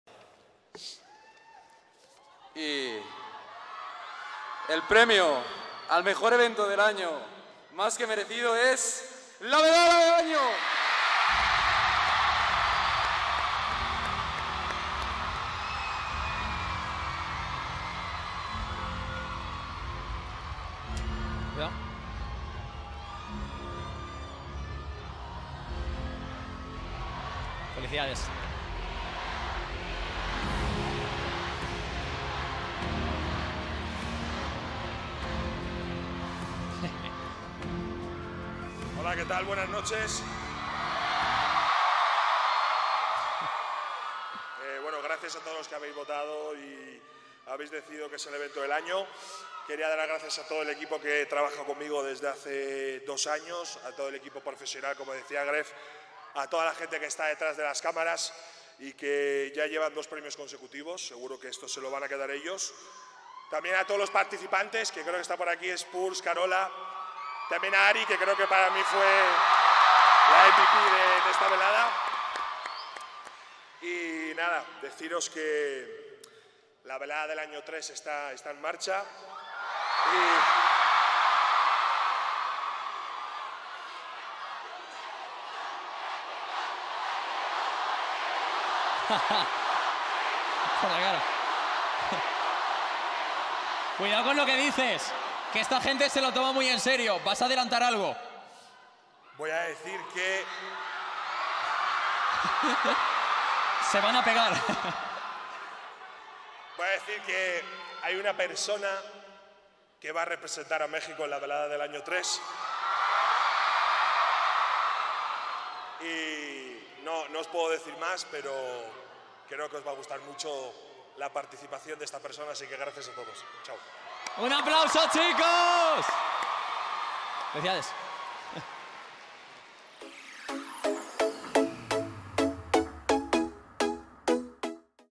VELADA DEL AÑO GANADOR EVENTO DEL AÑO 🥳 | PREMIOS ESLAND 2023
*El contenido de imágenes y clips de video son propiedad de los streamers.